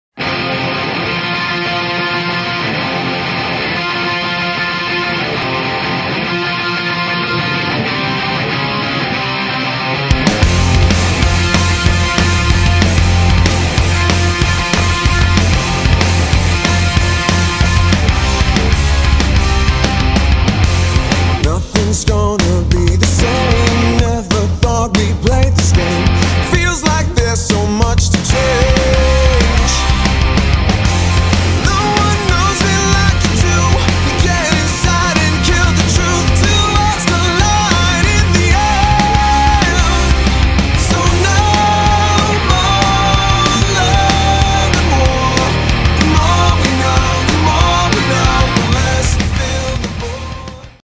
BMX
Fun off season training at the BMX track.